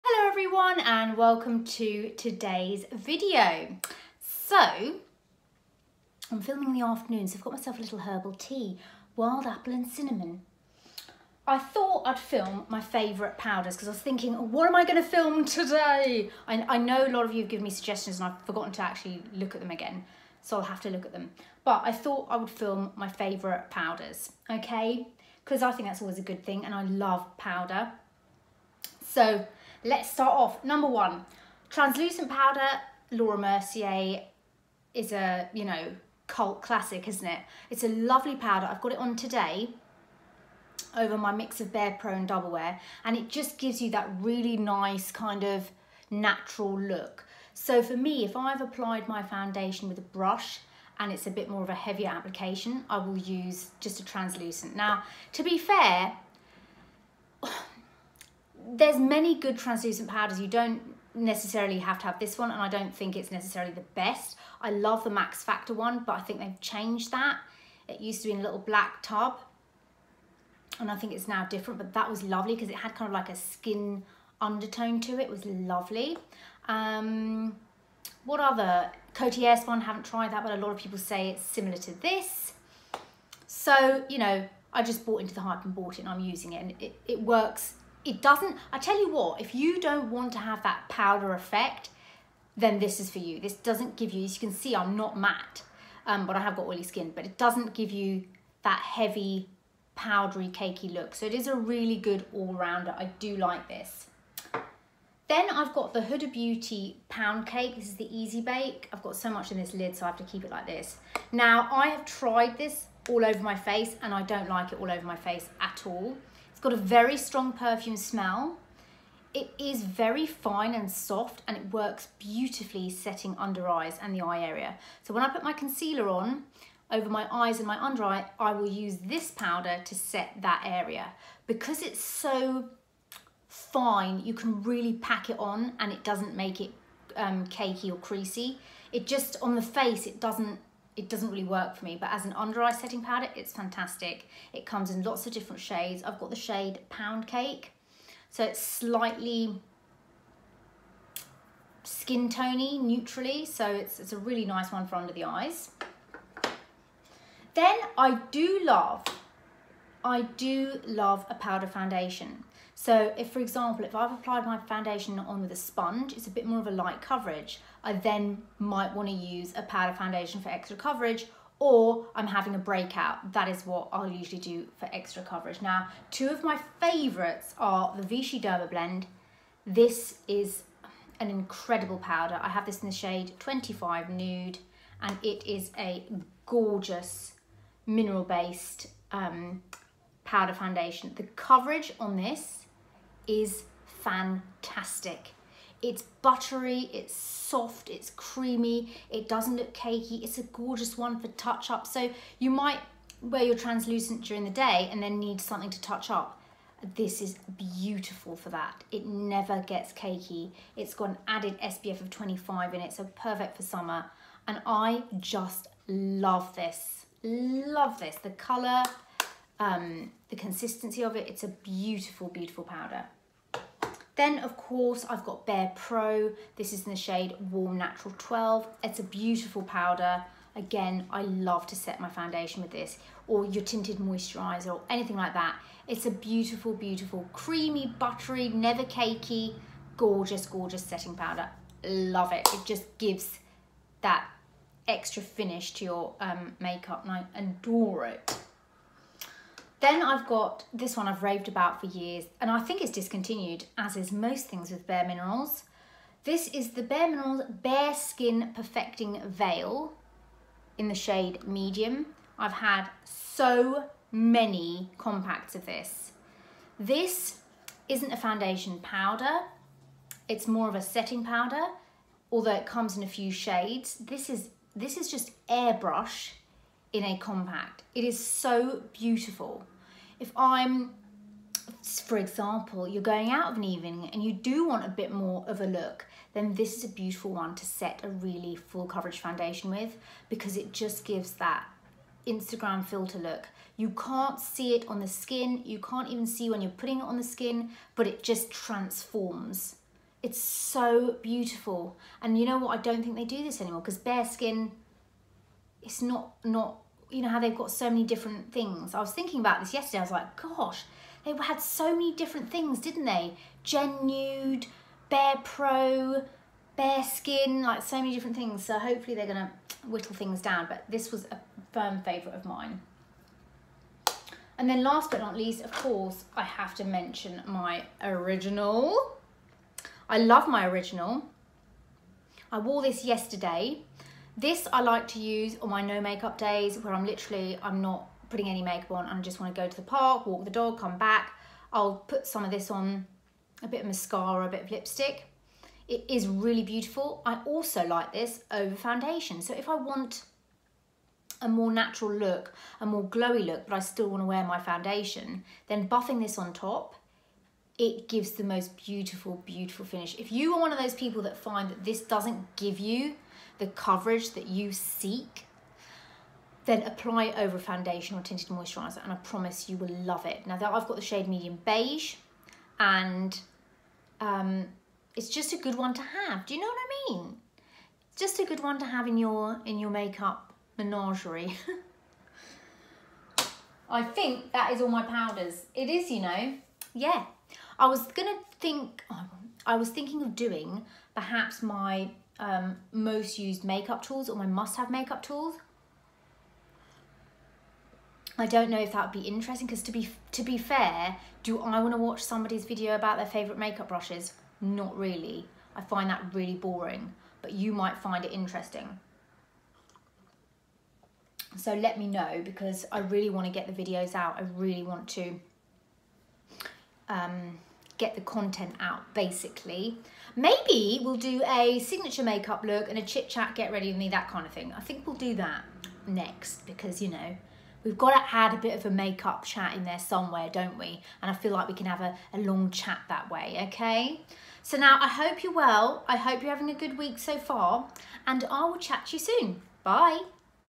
My Favourite Face Powders
My Jumper - Another Fox

VICHY Dermablend Covermatte Compact Powder Foundation -  25 Nude

BareMinerals BarePro Pressed Powder Foundation - warm natural 12

BareMinerals BareSkin Perfecting Veil - medium

BareMinerals Original - medium beige

Huda Beauty Easy Bake - Pound Cake

Here I share my love of makeup, beauty, and lifestyle.